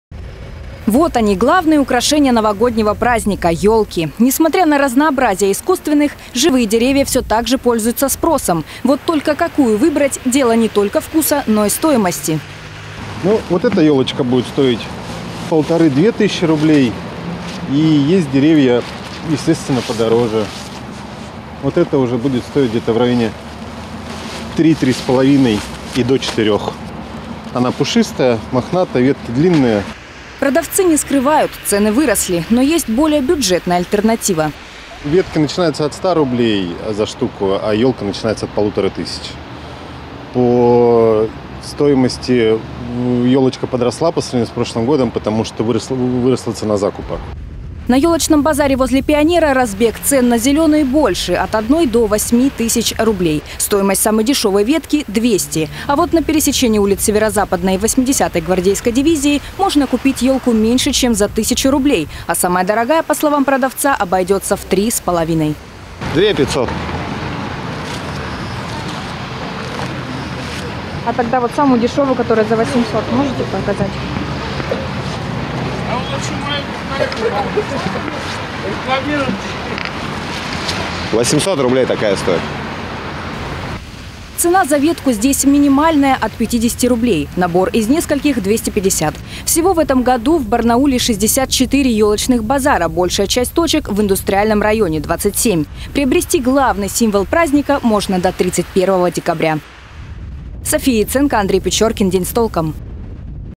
Какую выбрать – дело не только вкуса, но и цены. Сколько стоит лесная красавица в краевой столице
Барнаульцы продолжают готовиться к Новому году. Покупают подарки, продукты, украшают дома. Ну и, конечно же, обзаводятся главным символом праздника – ёлкой. О том, сколько стоит лесная красавица в краевой столице, – в сюжете Софии Яценко